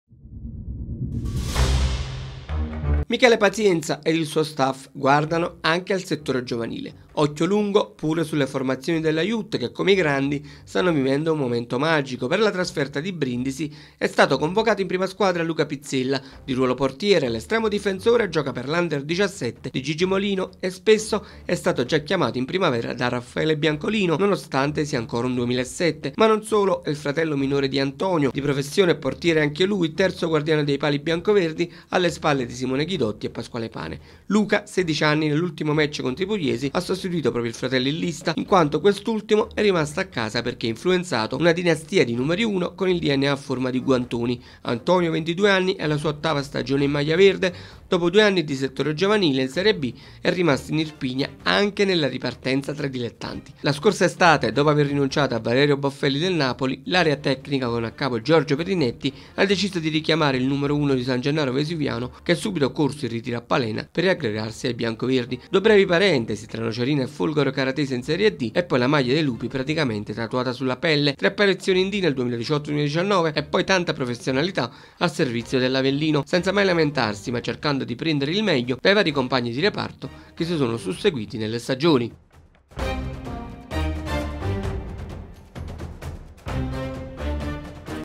Pizzella, dinastia di portieri a servizio dell'Avellino
Dopo Antonio un altro Pizzella per la porta dei lupi. Per Luca 16 anni contro il Brindisi prima convocazione con la squadra di Pazienza.